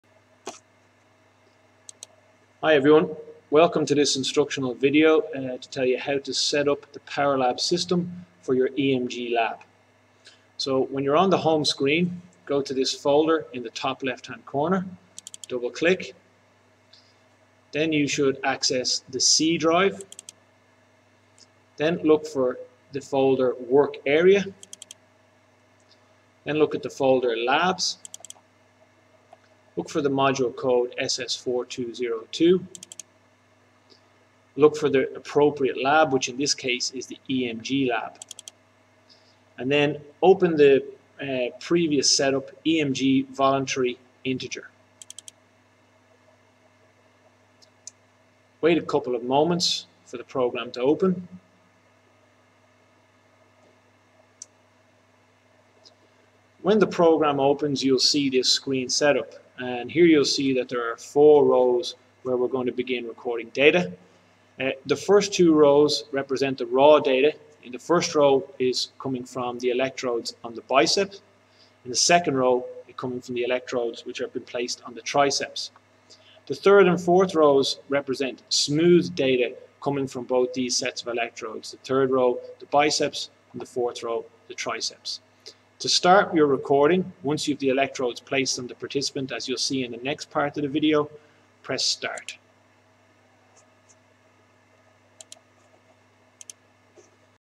EMG Lab Software Setup Part 1
Electromyography Lab (Dr Brian Carson) Part 1: Setup of LabChart Pro software for measurement of EMG